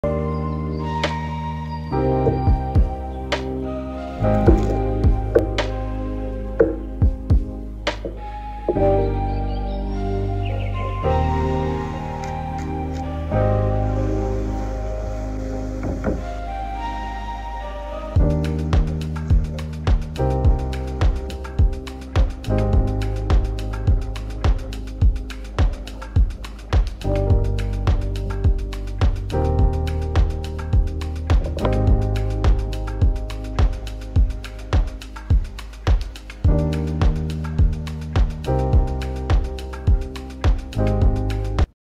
Portobello and Maitake One Pot Spaghetti (Vegan Gluten Free)
Sweat Onions, Saute mushrooms, in the same pot add spaghetti noodles and water. Keep on stirring to avoid sticking. The pasta water will help make the sauce creamy and became like. give this a tre, really easy and fun!

The noodles I used was Green Lentil (Low carp high protein)